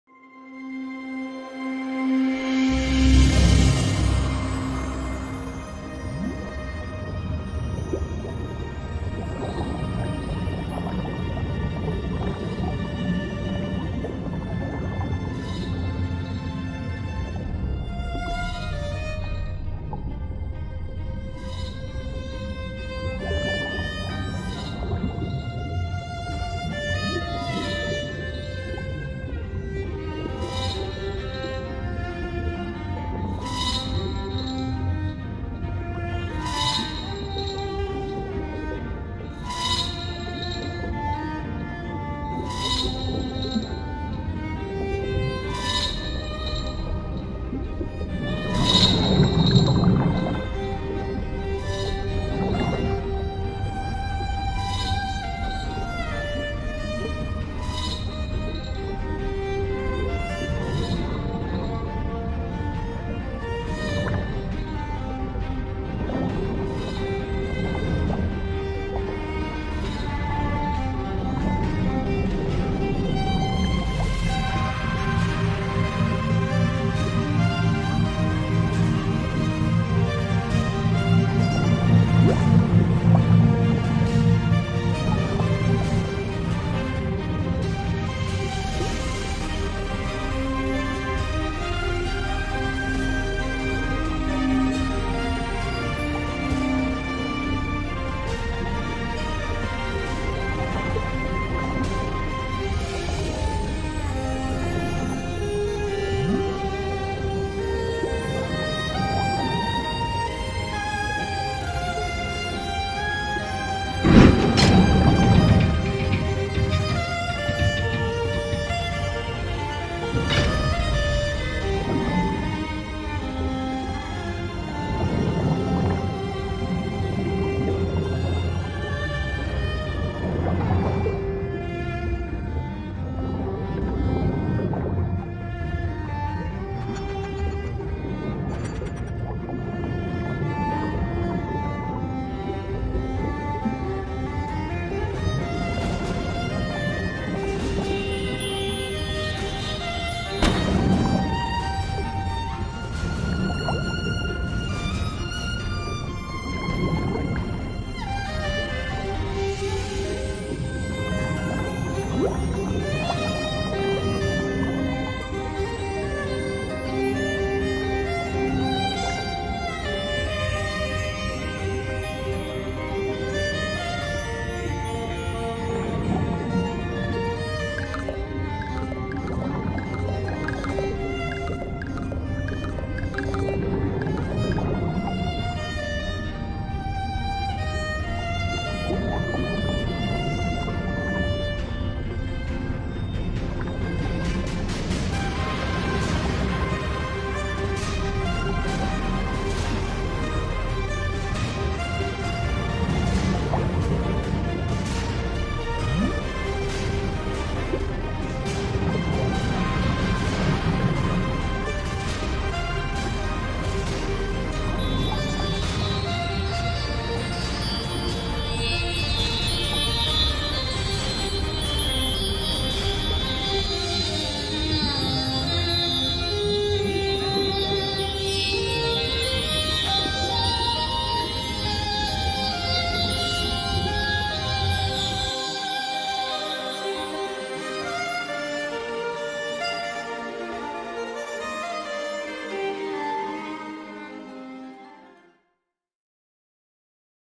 Mermaid's Turf - Animation 1 (Autodesk 3ds Max 2015)
The sea creatures built a turf in memory of the mermaid who died at young age. Mermaid's valuable items are kept here.